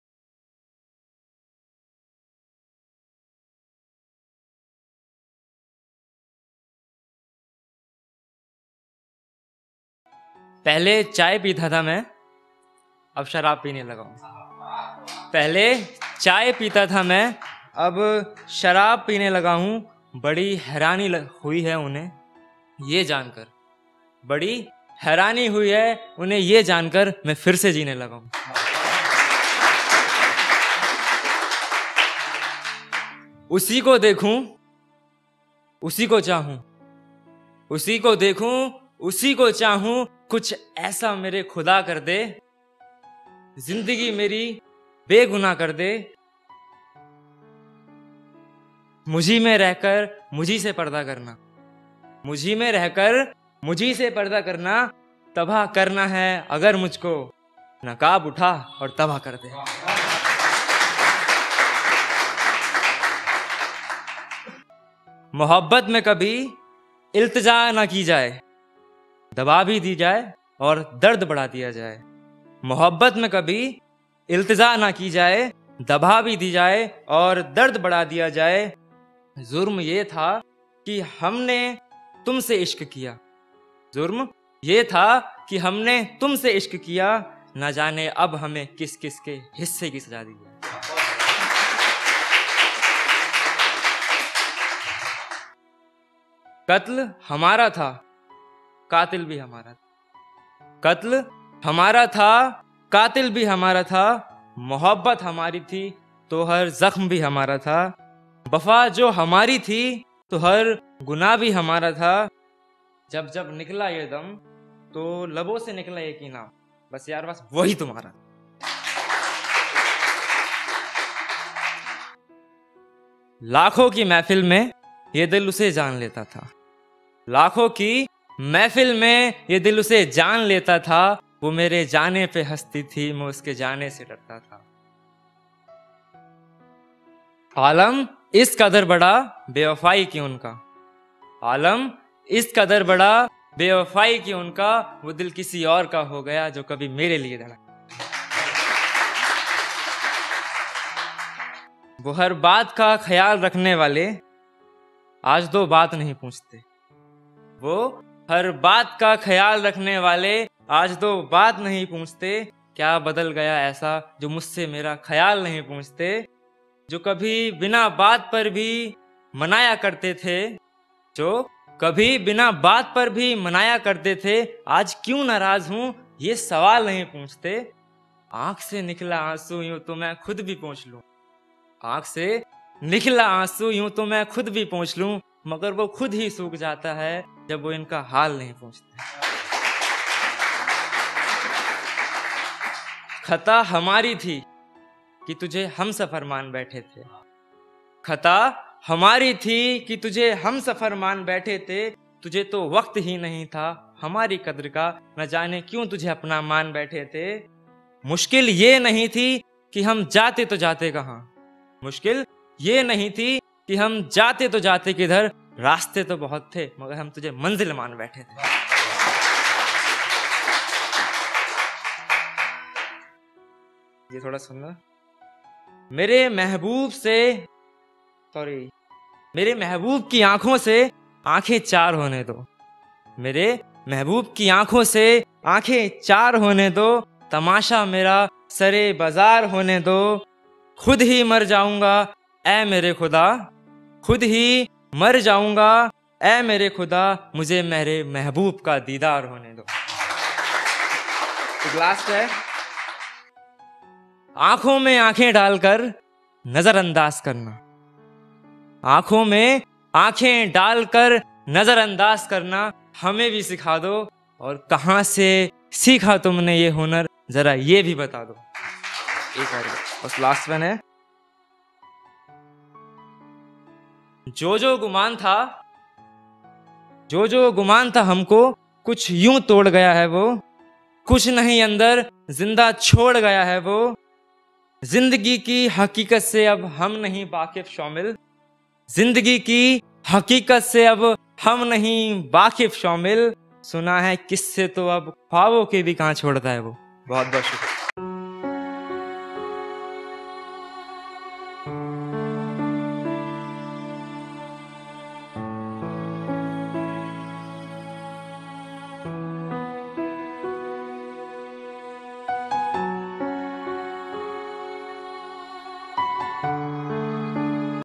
Dil Kya Tuta CHAI Se SHARAB Par Aa Gya Hoon | Poem by Nsomil Saxena| TRD Poetry | The Realistic Dice
TRD present poetry open mic in collaboration with ART BOX, metro Pillar Number 781, Land Mark Polo Garden, Dwarka Mod Metro Station, New Delhi. Thanks for having us ART BOX , venue was really amazing. If you are near by Dwarka and having interest interest in music and dance, do visit this place.